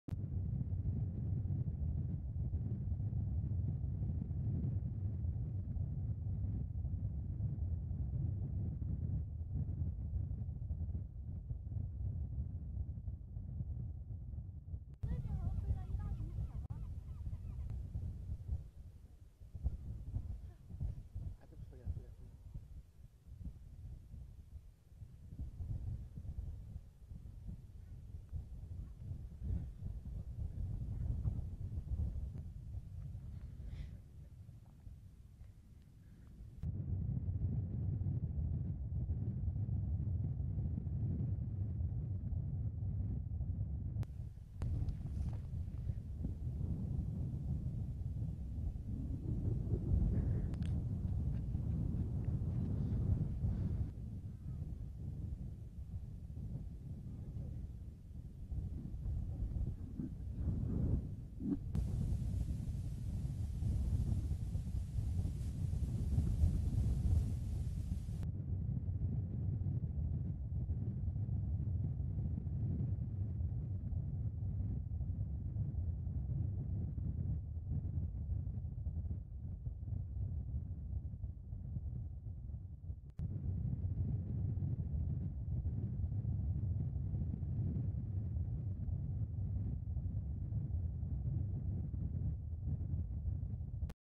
Over 1,800 White Storks Migrate to NE China
Over 1,800 white storks have migrated to the wetlands in Panjin City in northeast China’s Liaoning Province since this March as the weather has turned warm.